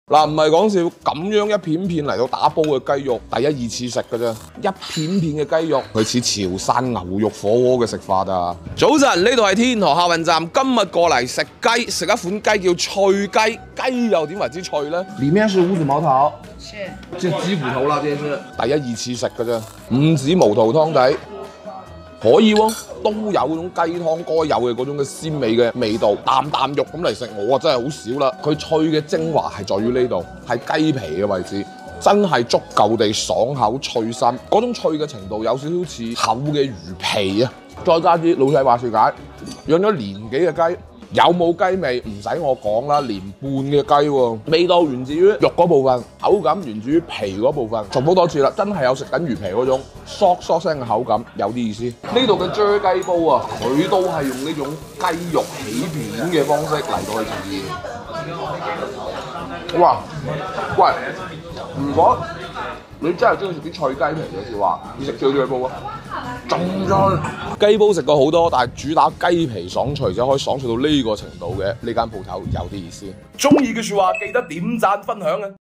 這種“脆皮雞”，我一個人能吃一整隻！肉嫩不柴，皮脆爽口~這種吃雞方式真的太愛了！【品城記】
這種“脆皮雞”，我一個人能吃一整隻！新鮮雞肉帶皮拆骨起片，再用山泉水浸熟或者生啫，肉嫩不柴，皮脆爽口~還能保持新鮮雞肉的甜香，這種吃雞方式真的太愛了！

吃雞！
品城記 廣東探店 雞煲 "我們將確保影片內容不涉及任何動物虐待行為，所有食材的取得都符合道德和法律規定。"